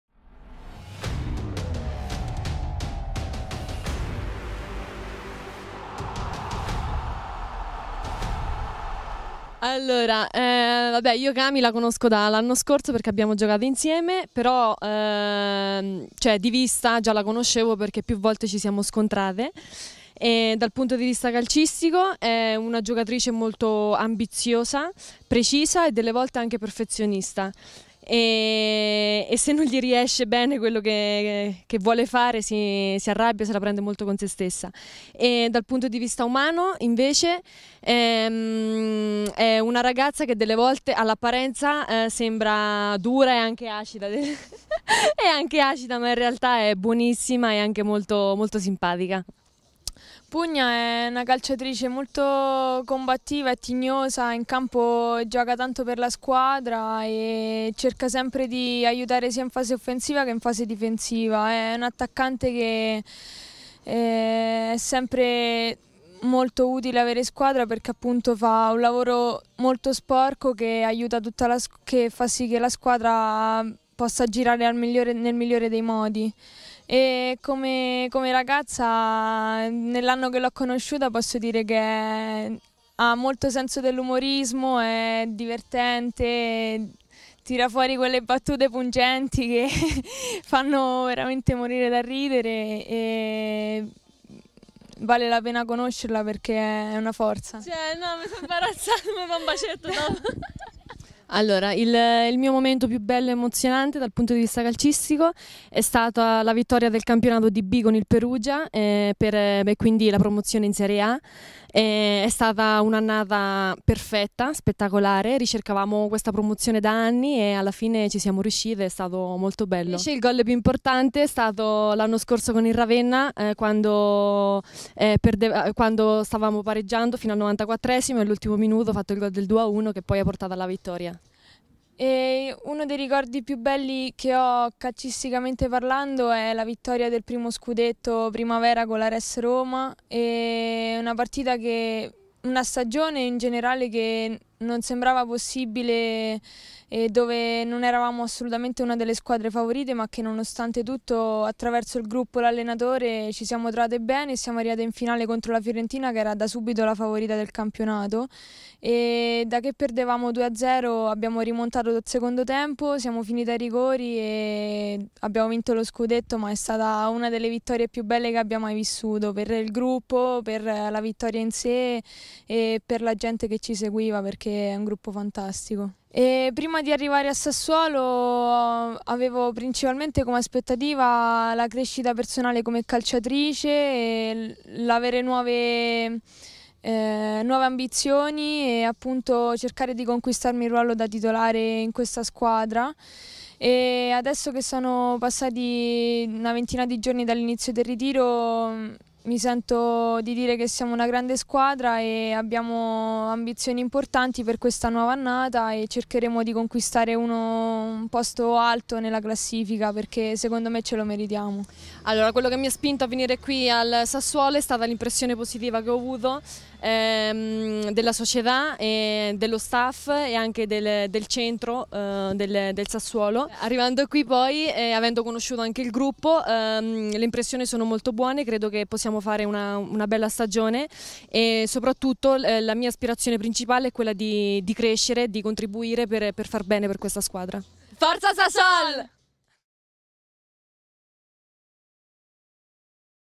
Sassuolo Femminile, le interviste a Camilla Labate e Luisa Pugnali
30 agosto 2019 - conosciamo meglio le calciatrici neroverdi Camilla Labate e Luisa Pugnali